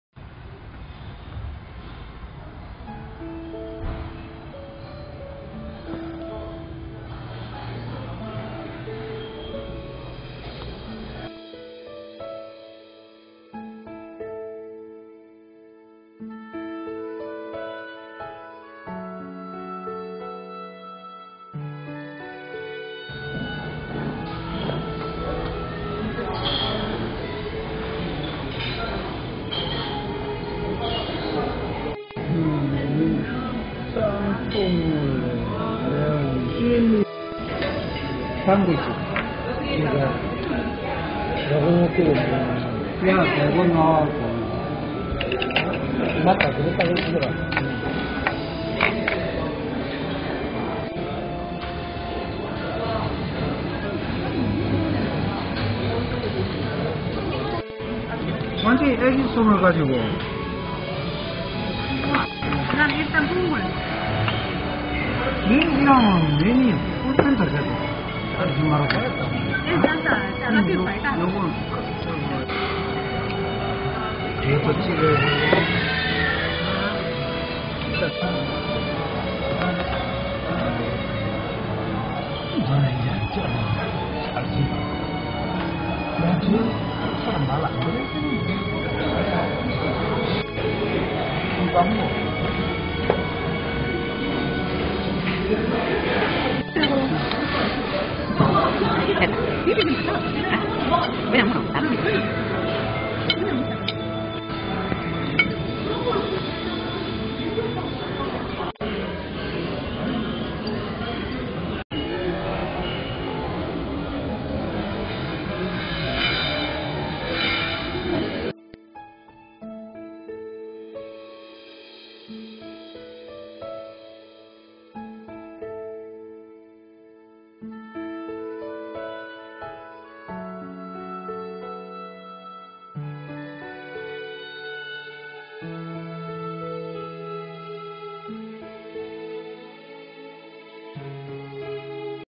다시 찾고 싶다는 맛, 자금성
좀처럼 외식을 하지 않는 남편
"오늘 우리 형님하고 자금성 갈까?"
"당신 중국음식 싫어하잖아!"
"자금성에 갈 일이 좀 있어서"
집에서 밥 안먹는다는데
형부네와 얼른 따라 나섰습니다.